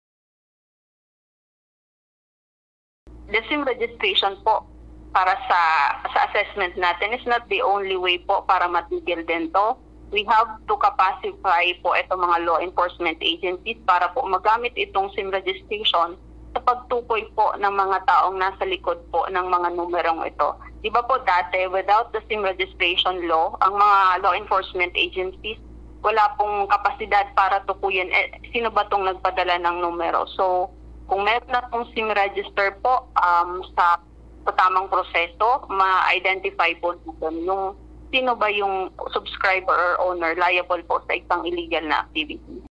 𝐏𝐡𝐨𝐧𝐞 𝐧𝐮𝐦𝐛𝐞𝐫𝐬 𝐮𝐬𝐞𝐝 𝐟𝐨𝐫 𝐬𝐜𝐚𝐦 𝐦𝐞𝐬𝐬𝐚𝐠𝐞𝐬 𝐜𝐚𝐧 𝐛𝐞 𝐝𝐢𝐫𝐞𝐜𝐭𝐥𝐲 𝐛𝐥𝐨𝐜𝐤𝐞𝐝 𝐛𝐲 𝐍𝐓𝐂 𝐁𝐢𝐜𝐨𝐥
𝐇𝐢𝐥𝐢𝐧𝐠𝐨𝐧 | Pwedeng direkta ng ipa-block asin irani sa National Telecommunications Commission Bicol an mga numero na nagse-send nin mga scam messages.

//AG • Tune in for more updates